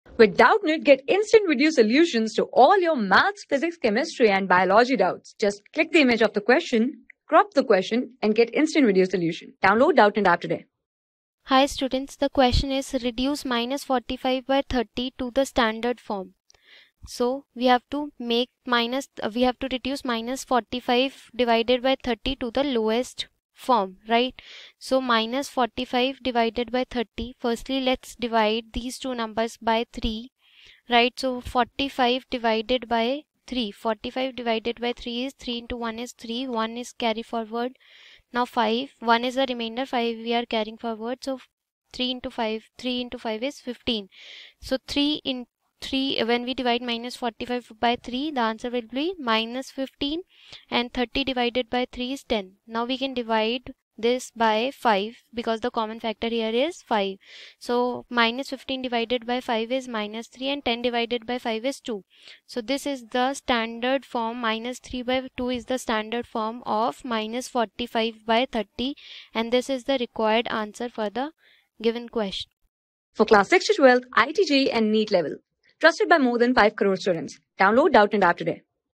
Reduce (-45)/30 to the standard for | 7 | RATIONAL NUMBERS | MATHS | NCERT ENGLISH | Doubtnut
Reduce (-45)/30 to the standard for
Class: 7
Subject: MATHS
Chapter: RATIONAL NUMBERS
Board:CBSE

👉 Have Any Query? Ask Us.